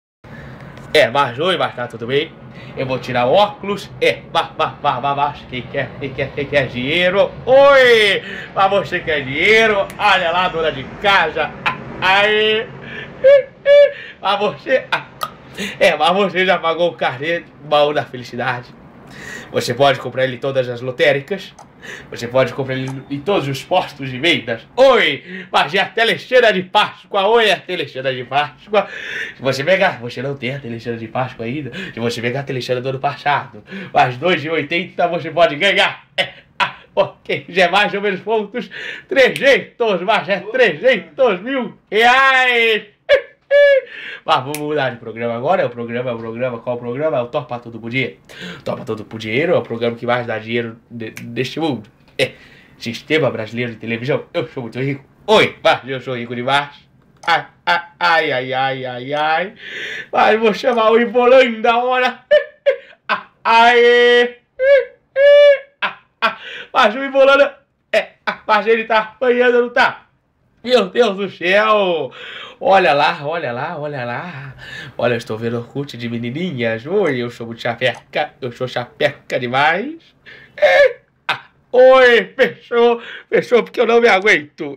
Ericão SILVIO OEEEEE!
Cidadão fica trancado em SAMPA estudando é lógico que uma hora PIRA né!

Mas o cara manda beeeem no Silvio Santos, centro a alegria oeee!